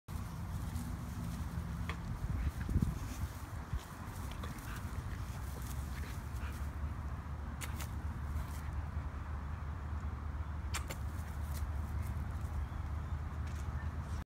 Roscoe loving life
Roscoe the day he was rescued from a freeway near downtown Houston